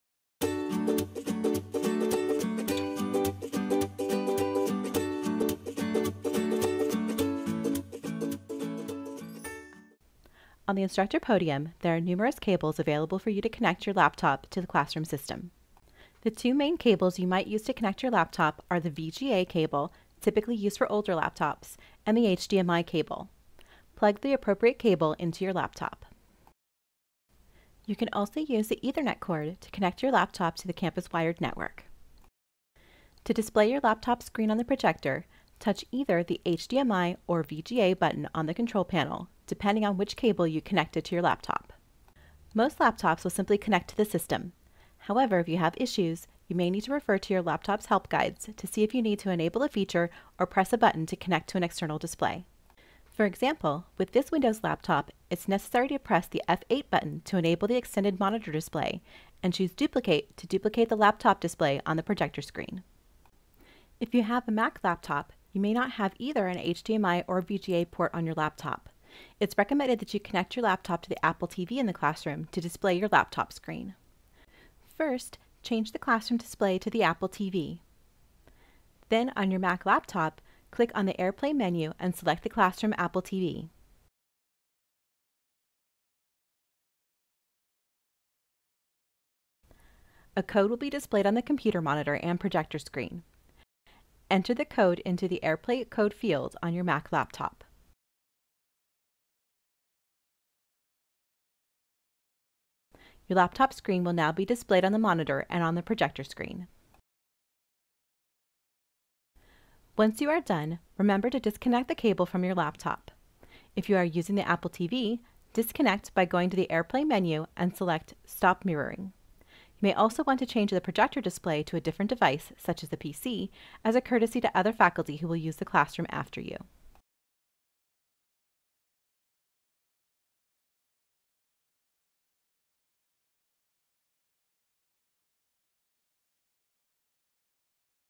Using Your Laptop in the Classroom (General Use Classroom)
How to connect your laptop to the classroom system using an HDMI/VGA cable or the AppleTV if your laptop does not have HDMI/VGA ports.